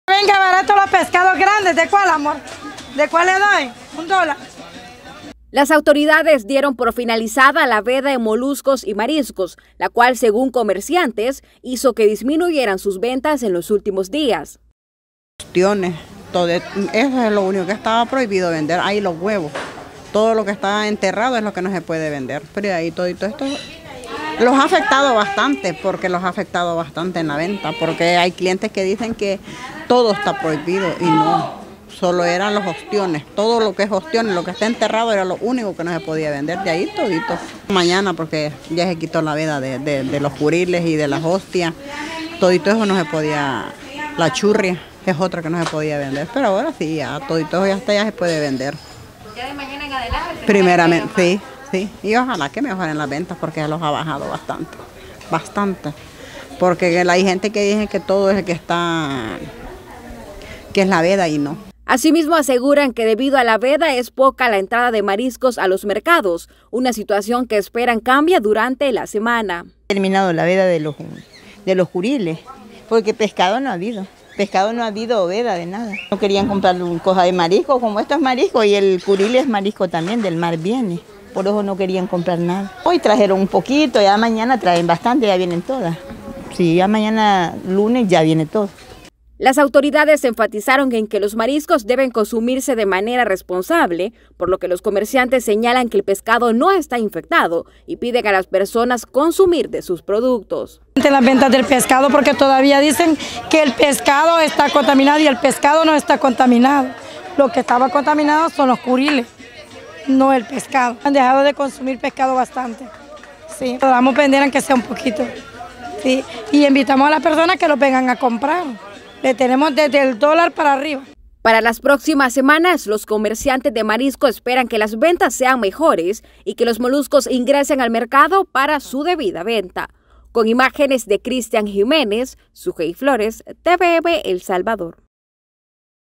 Afectaciones por la veda de moluscos en San Miguel
En el departamento de San Miguel, comerciantes aún no se recuperan de las pérdidas que tuvieron a raíz de las afectaciones que dejó la veda de moluscos y esperan mejorar sus ventas en los próximos días.